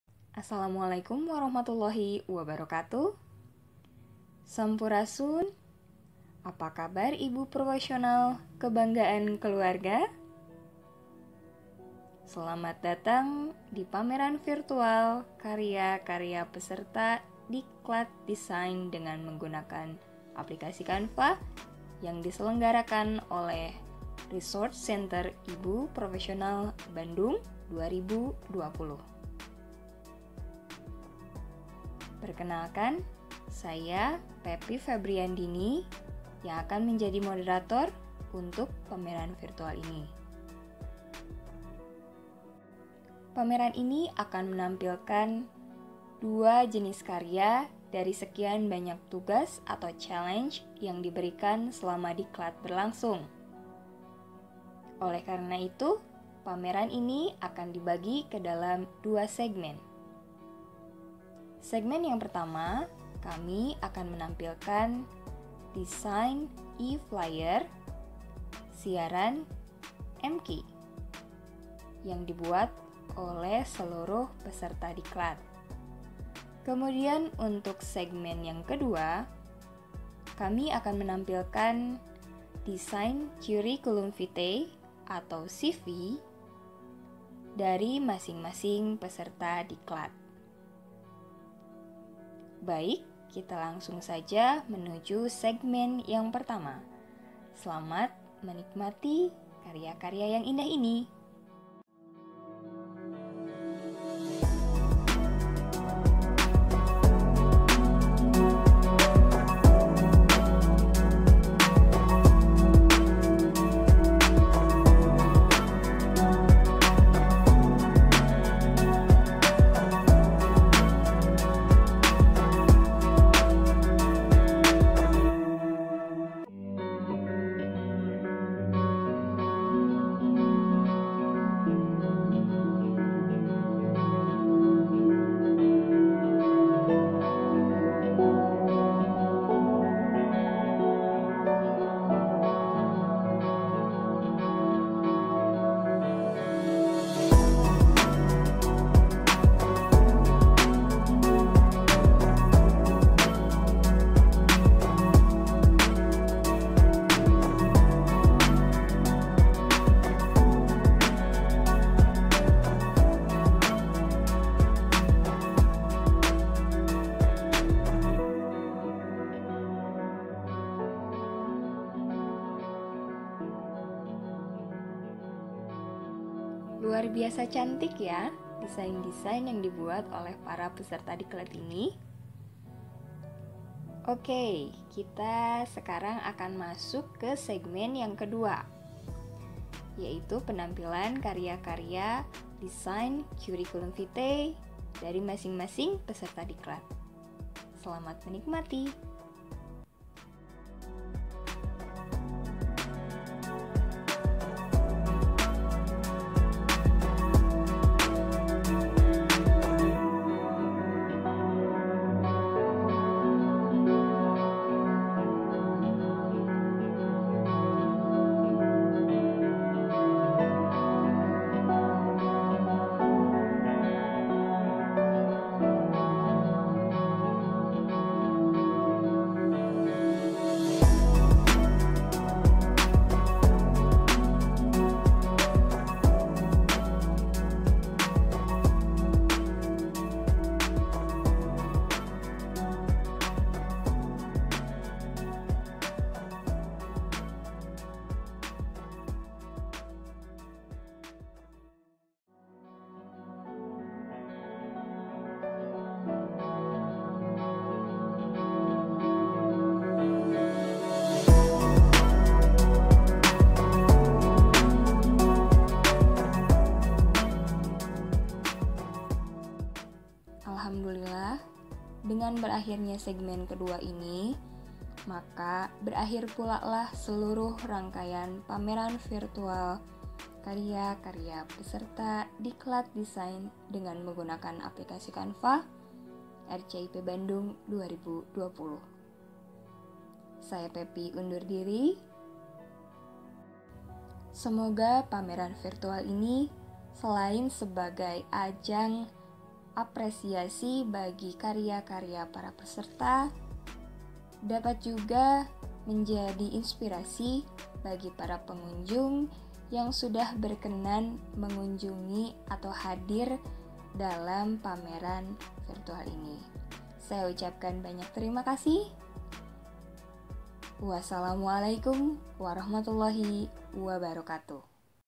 PAMERAN VIRTUAL Karya Peserta Diklat Desain | RCIP BANDUNG 2020
Selamat datang di PAMERAN VIRTUAL Karya Para Peserta Diklat Desain Grafis dengan Aplikasi CANVA yang diselenggarakan oleh RCIP Bandung selama bulan Juni hingga awal Agustus 2020.

Silakan "berkeliling" menikmati karya-karya desain para peserta dengan menggunakan aplikasi CANVA ini.